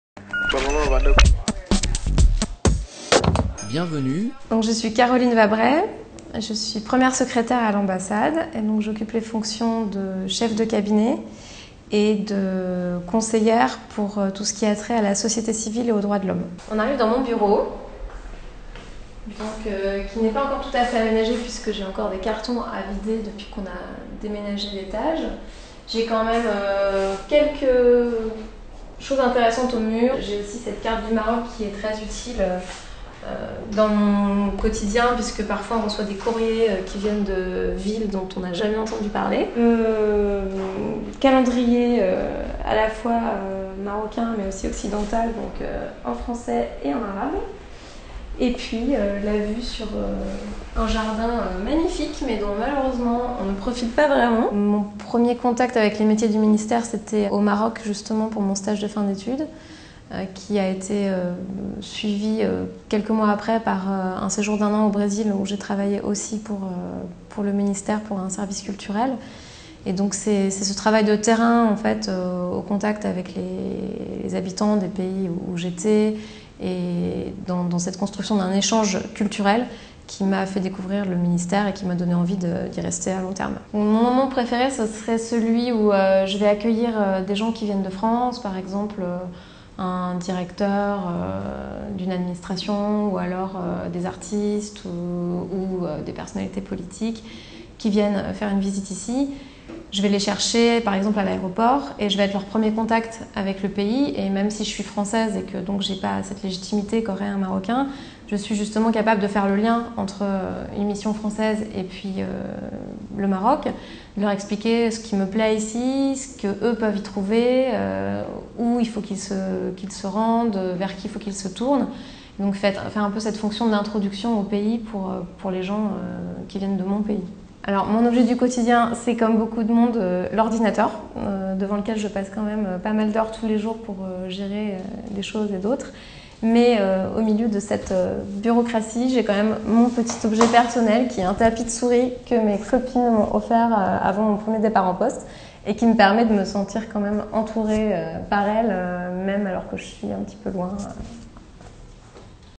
CAROLINE VABRET
Ils font les affaires étrangères, pourquoi pas vous ?
De Paris, en passant par Nantes, Dakar, ou Rabat, les personnes qui travaillent aux Affaires étrangères ont tous des points communs :
Des bureaux pas super sexy, un objet du quotidien, un moment préféré et surtout plusieurs vies en une.
Les 23 portraits permettent à travers des personnalités d’avoir un aperçu des différentes carrières, de la richesse des parcours, des multiples pays et missions qu’offre la diplomatie. Les portes sont donc ouvertes à celles et ceux qui ont envie de vivre plusieurs vie en une.

Instragram : @francediplo